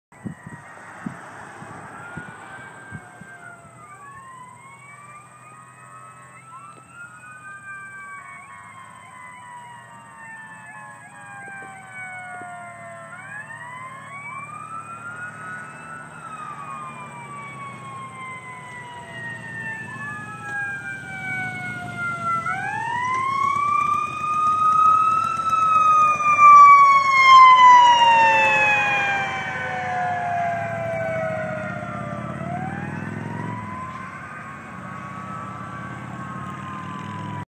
Medic 341 Responding with Police Escort
Little Rock-Fox Medic 341 responding to the hospital. Plano PD escorted them to the edge of the city limits.